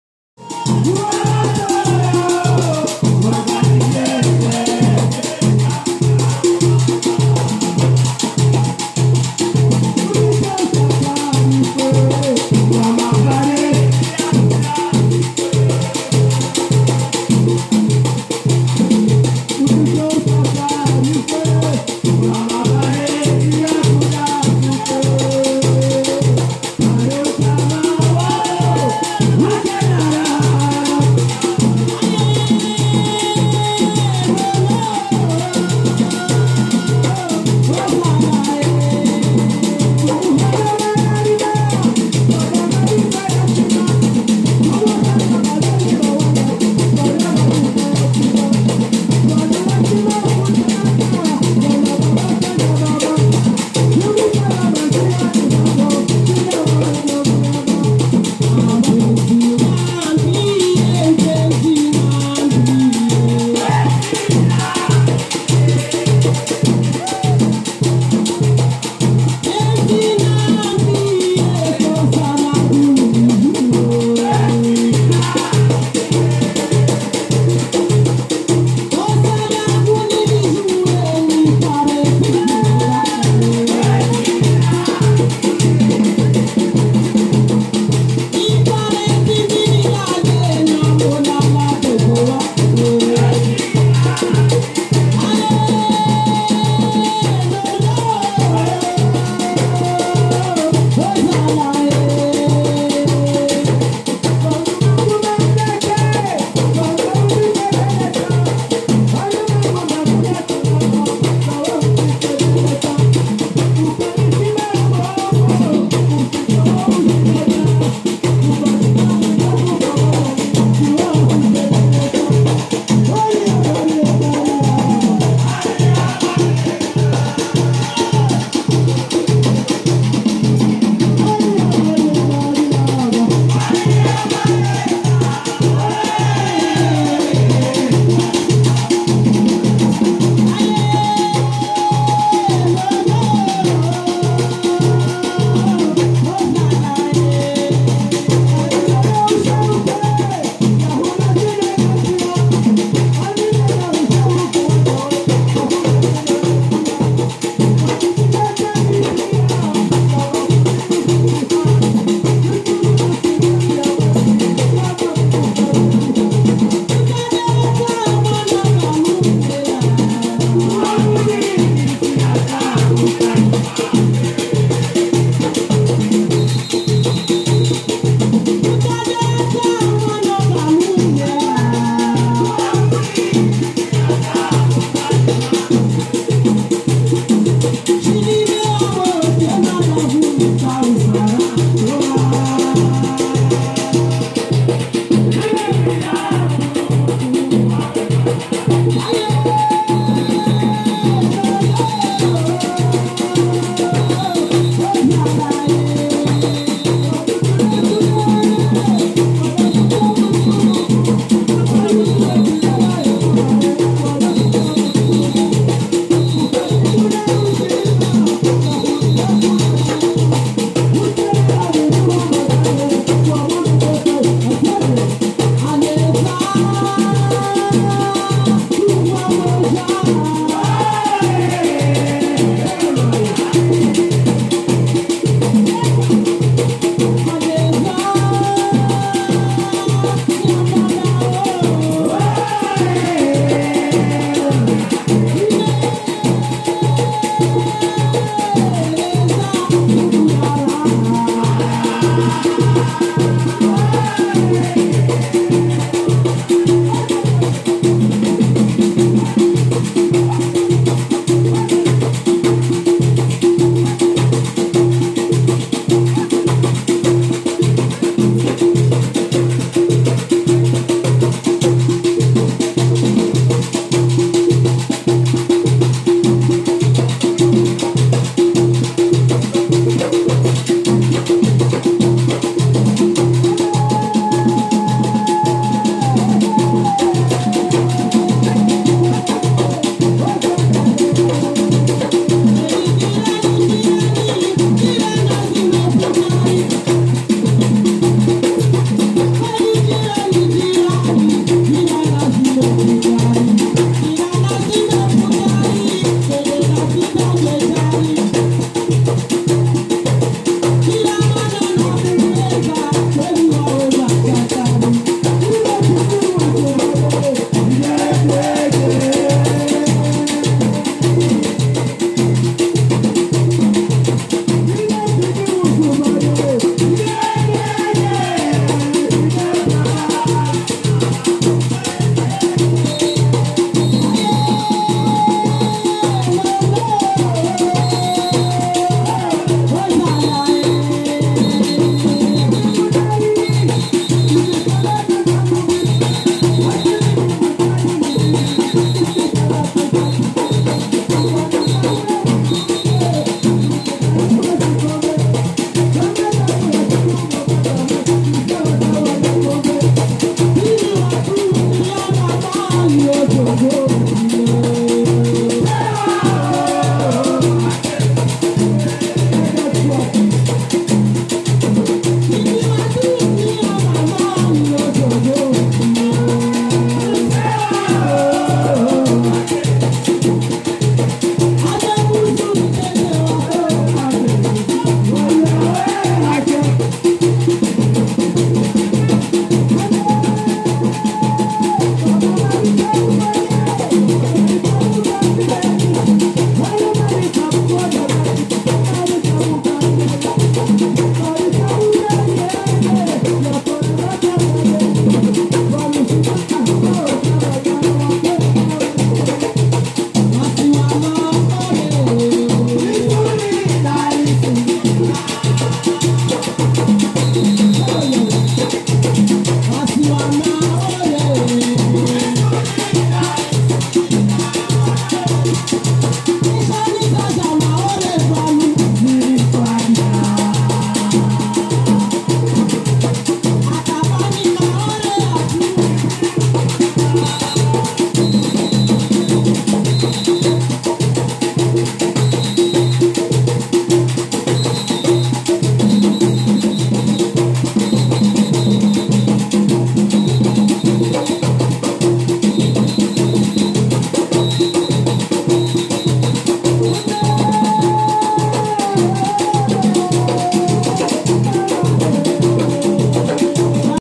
Chigoma à nyambadao Mayotte . outamadouni watrou 2016 . Meilleur danse de chigoma
Danse traditionnelle Maoraise qui se fait à l'occasion  des mariage. Ici une jeune femme accompagne le chanteur. C'est un Groupe qui vient tout de droit du nord de Mayotte , de Mtsaha plus exactement